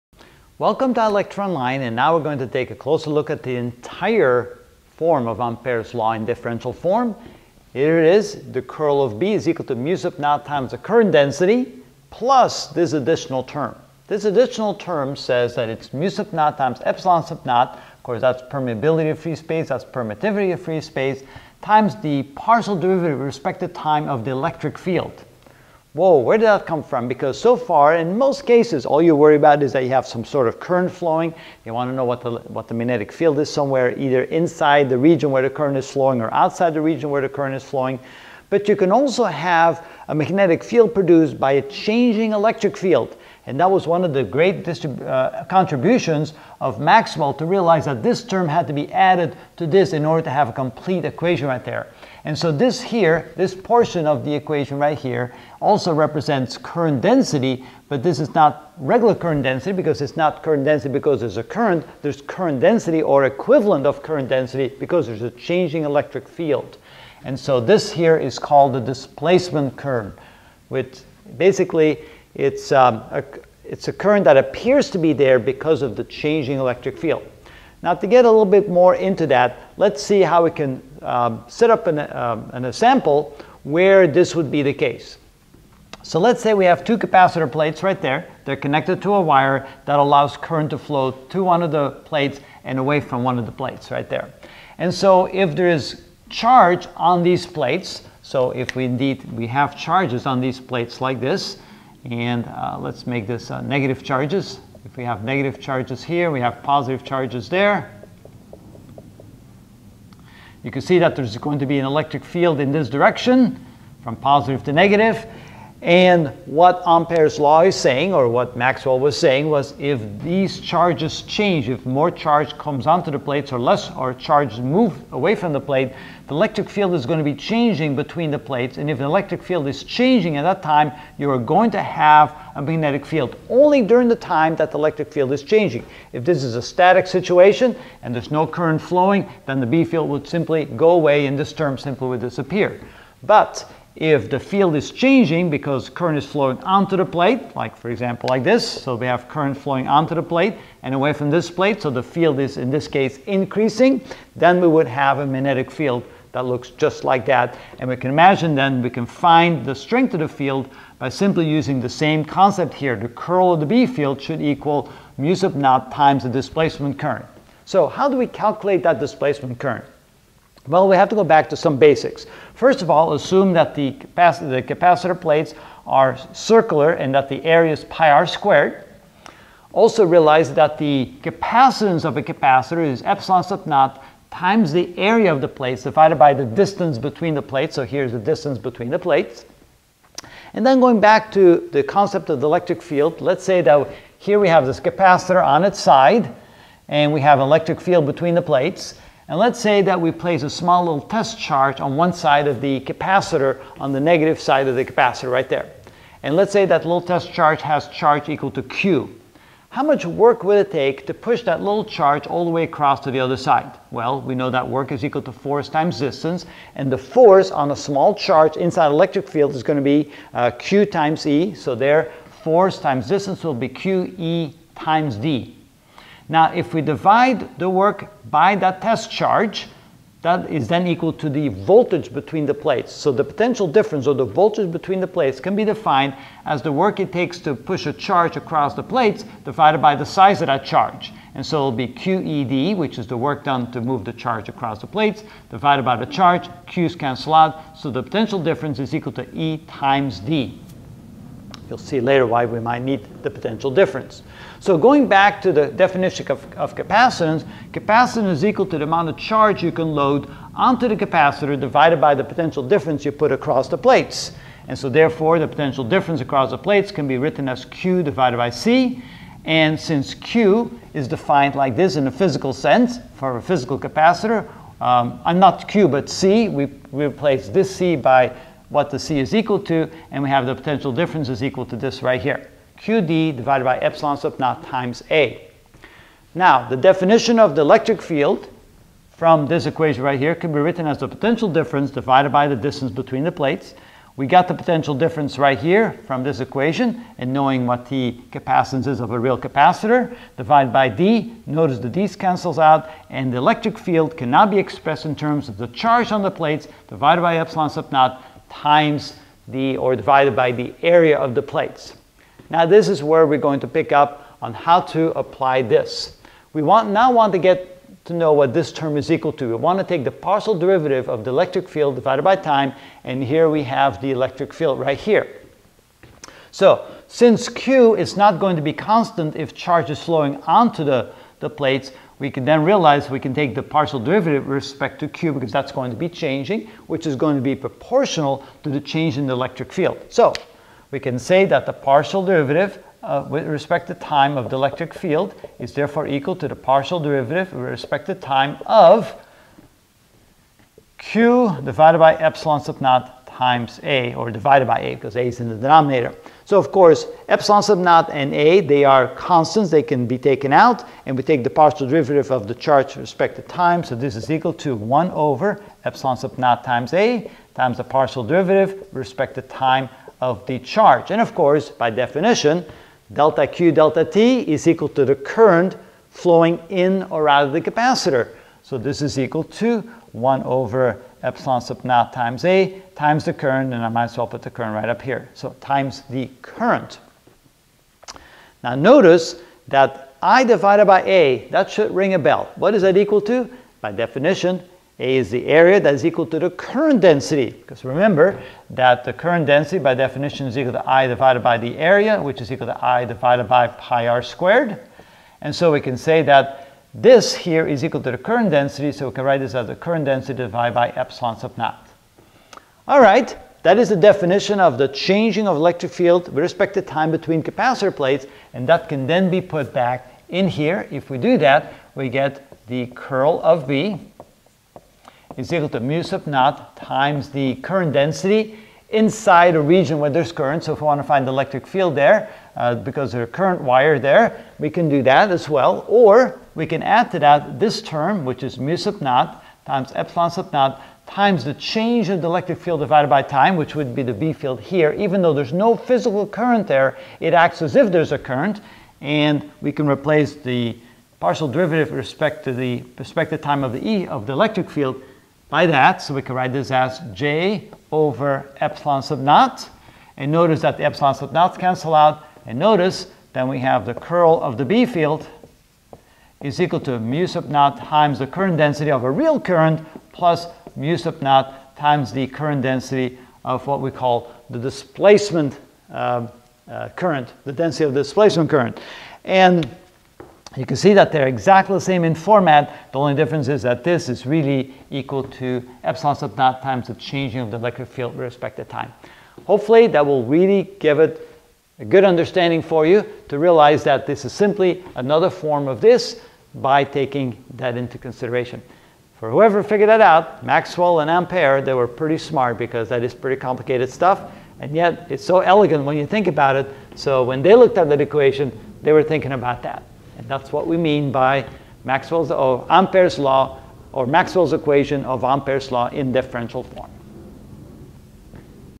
Physics 46 Maxwell's Equations (27 of 30) Differential Form of Ampere's Law: Displacement Current
In this video I will explain the entire form of Ampere's Law in differential form.